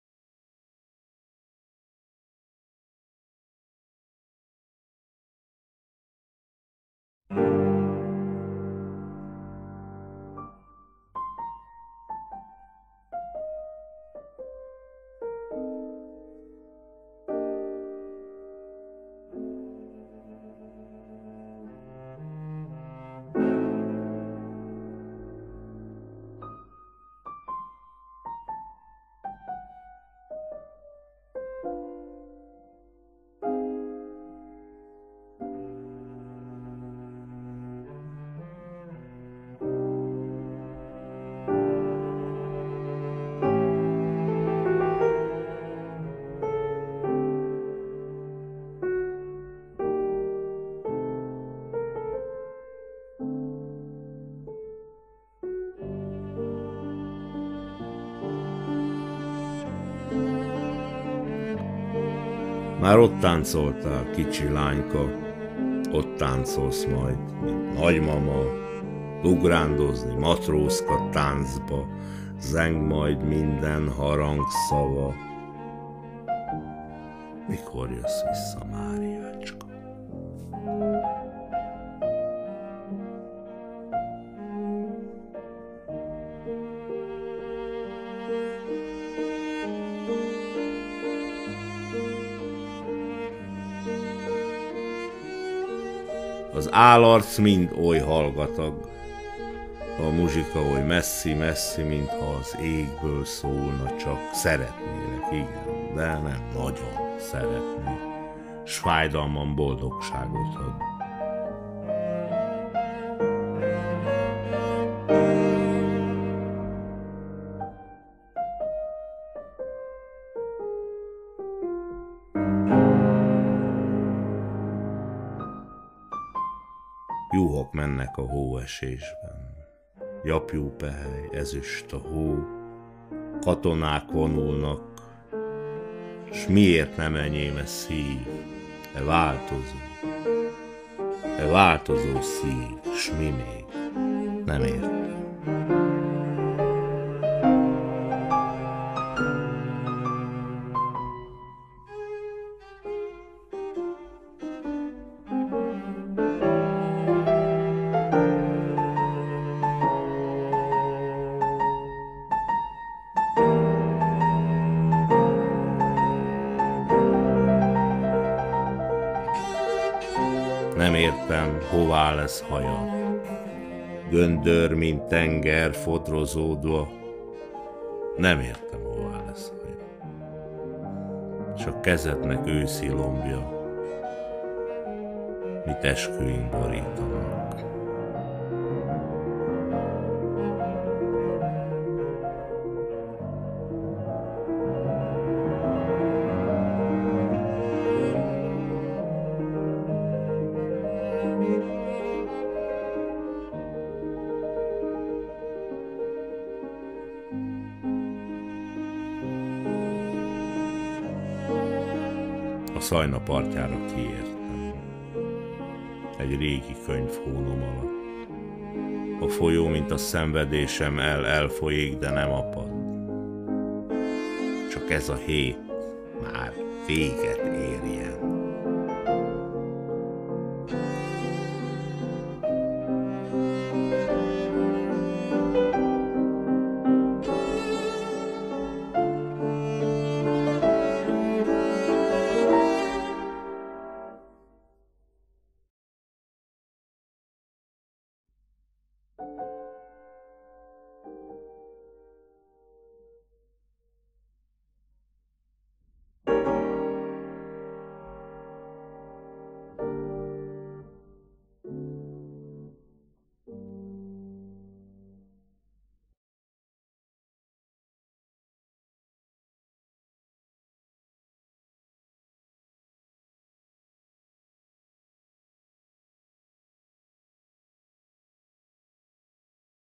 Mary / Marie
Directed: P.T.

Máriácska

Már ott táncoltál kicsi lányka
Ott táncolsz majd mint nagymama
Ugrándozni matrózka-táncba
Zeng majd minden harang szava
Mikor jössz vissza Máriácska

Az álarc mind oly hallgatag
A muzsika oly messzi messzi
Mintha az égből szólna csak
Szeretnélek igen de nem nagyon szeretni
S fájdalmam boldogságot ad

Juhok mennek a hóesésben
Gyapjúpehely ezüst a hó
Katonák vonulnak s miért nem
Enyém e szív e változó
Változó szív s mi még nem értem

Nem értem hova lesz hajad
Göndör mint tenger fodrozódva
És a kezednek őszi lombja
Mit esküink borítanak

A Szajna partjára kiértem
Egy régi könyv hónom alatt
A folyó mint a szenvedésem
El-elfolyik de nem apad
Csak ez a hét már végetérjen

G. Apollinaire - Marie

Vous y dansiez petite fille
Y danserez-vous mère-grand
C'est la maclotte qui sautille
Toute les cloches sonneront
Quand donc reviendrez-vous Marie

Les masques sont silencieux
Et la musique est si lointaine
Qu'elle semble venir des cieux
Oui je veux vous aimer mais vous aimer à peine
Et mon mal est délicieux

Les brebis s'en vont dans la neige
Flocons de laine et ceux d'argent
Des soldats passent et que n'ai-je
Un cœur à moi ce cœur changeant
Changeant et puis encor que sais-je

Sais-je où s'en iront tes cheveux
Crépus comme mer qui moutonne
Et tes mains feuilles de l'automne
Que jonchent aussi nos aveux

Je passais au bord de la Seine
Un livre ancien sous le bras
Le fleuve est pareil à ma peine
Il s'écoule et ne tarit pas
Quand donc finira la semaine